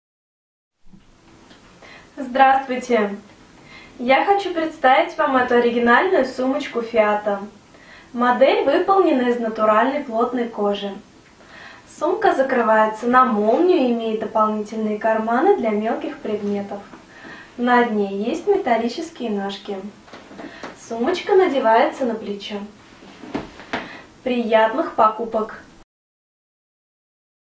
Сумка Fiato 3161 green black
Эта сумочка придется по вкусу женщинам, которые стремятся быть выразительными, не отказывая себе в комфорте. Великолепная модель из натуральной кожи. Стильная и очень удобная, прекрасно подойдет на каждый день. Сумочка украшена золотистой фурнитурой. Закрывается на молнию, носится на сгибе руки или надевается на плечо. На дне имеются металлические ножки. Помещаются документы формата А4. Держит форму.
Внутри: два отдела, разделенных карманом на молнии. На задней стенке карман для документов на молнии, на передней стенке карман для мобильного телефона и карман для мелочей.
Снаружи: на задней части карман для документов на молнии.